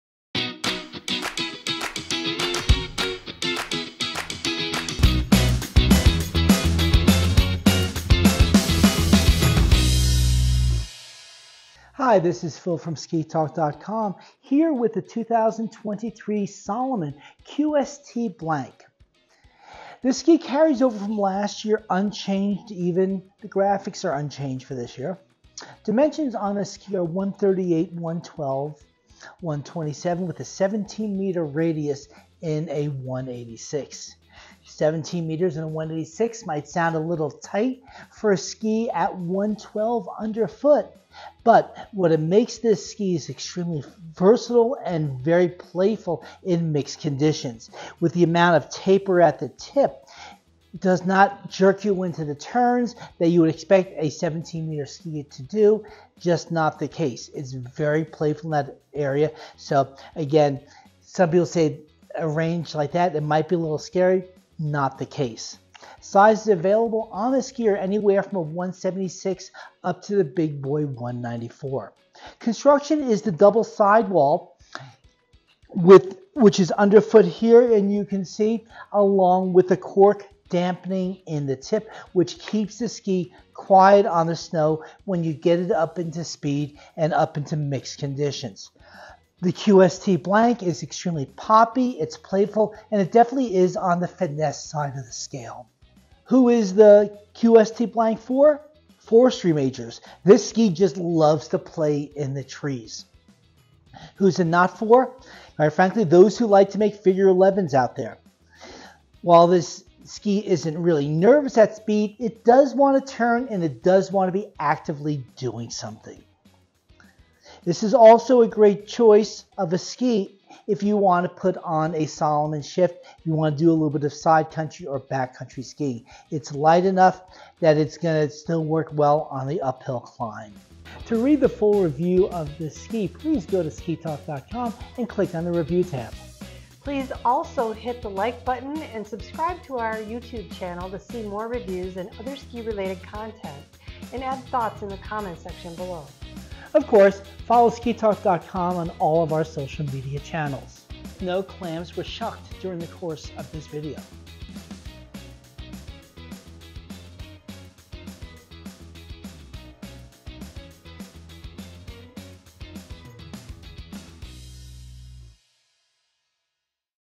2023 Salomon QST Blank Ski Review from SkiTalk.com
Phil Pugliese talks about the Salomon QST Blank Skis.
If you enjoyed this review, you can read the full written review on SkiTalk.com